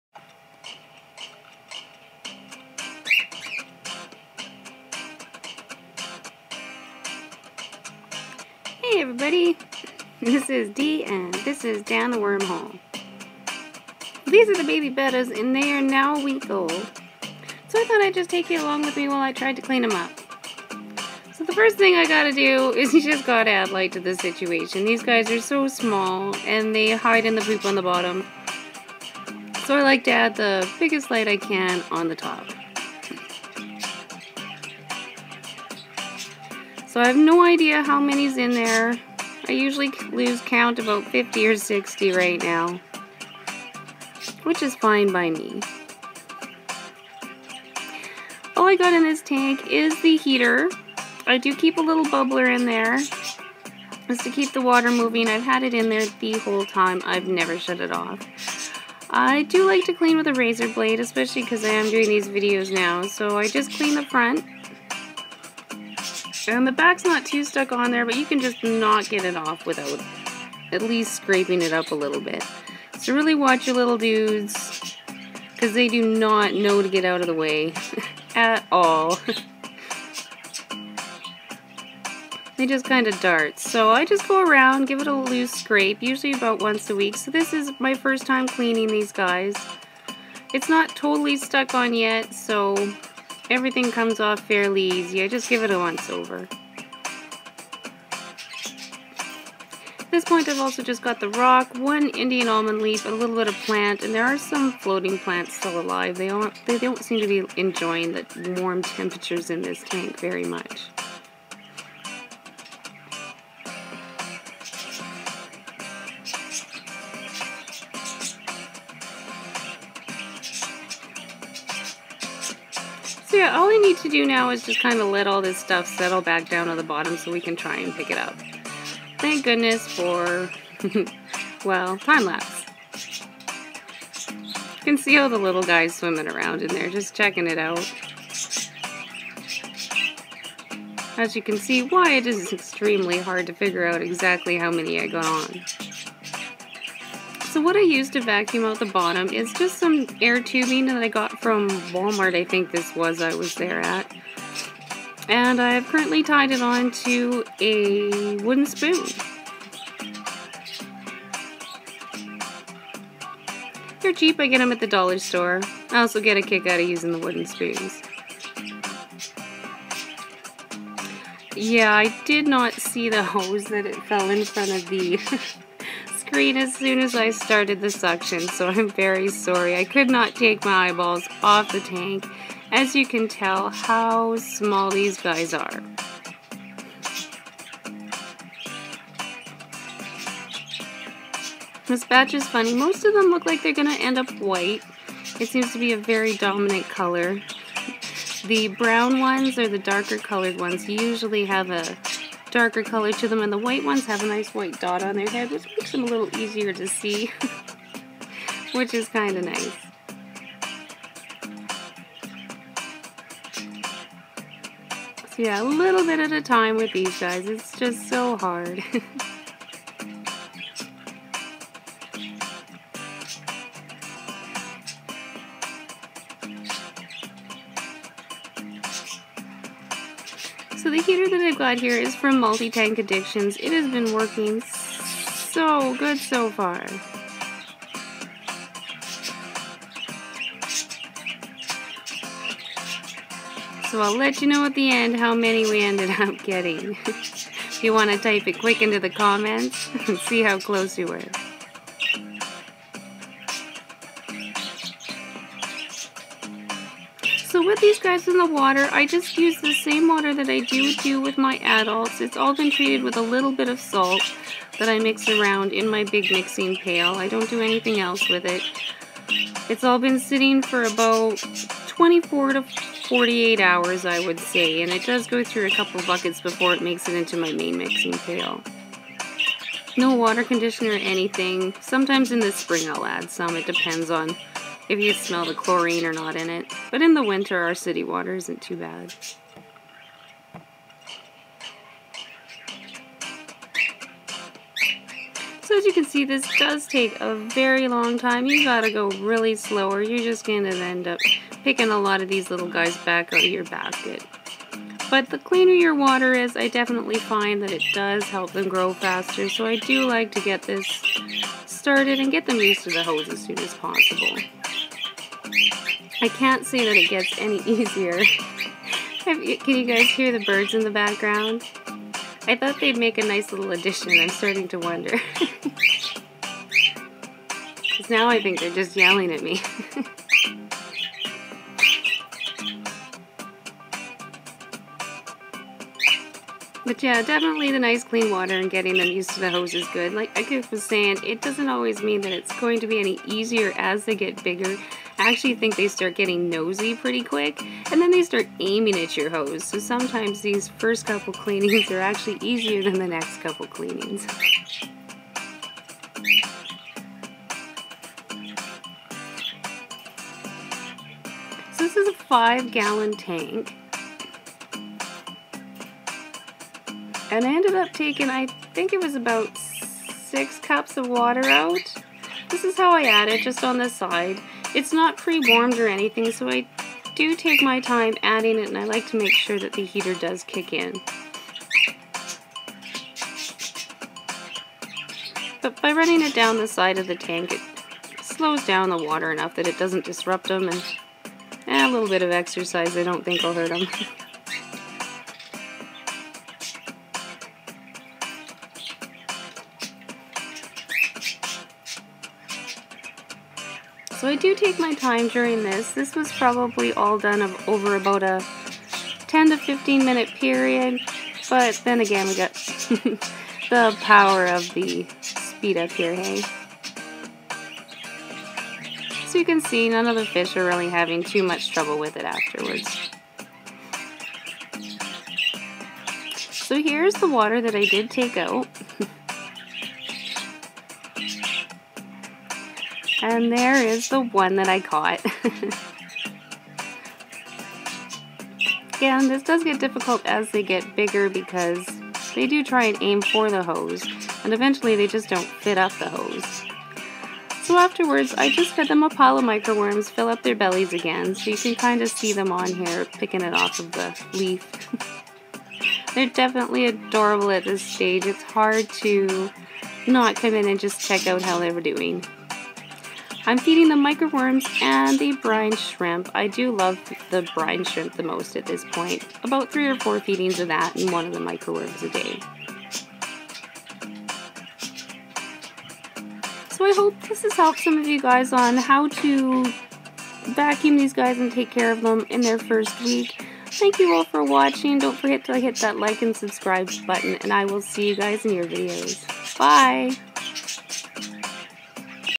Cleaning baby bettas
How I clean up the baby bettas,  one week old care.  part of the grow along hoping for some dumbo babies this time.  Thank you as always for watching!